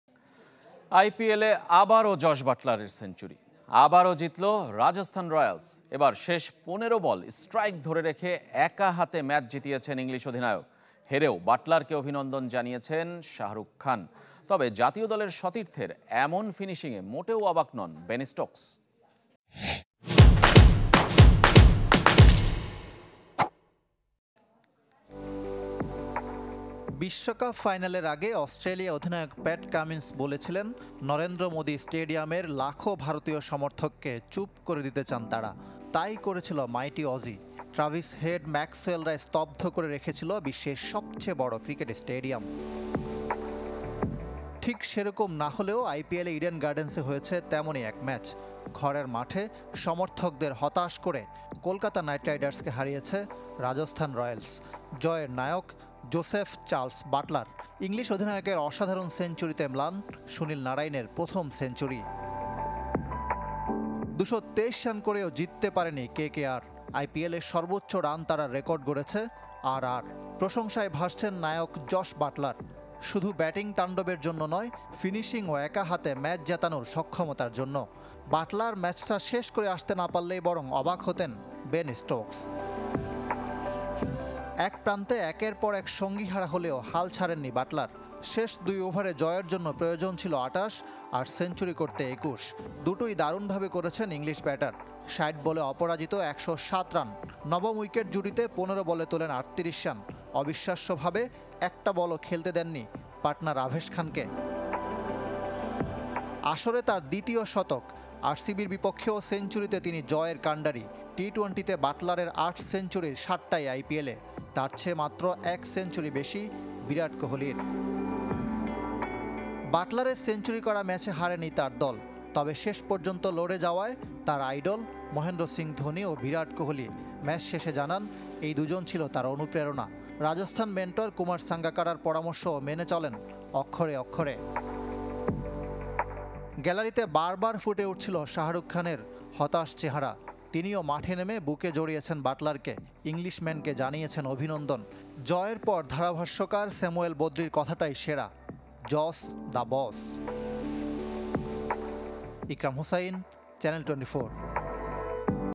কলকাতাকে স্তব্ধ করে বাটলারের সেঞ্চুরিতে রাজস্থানের জয় | Jos Buttler | IPL 2024 | Channel 24
কলকাতাকে স্তব্ধ করে বাটলারের সেঞ্চুরিতে রাজস্তানের জয়

»» Office Address:
Bangladesh.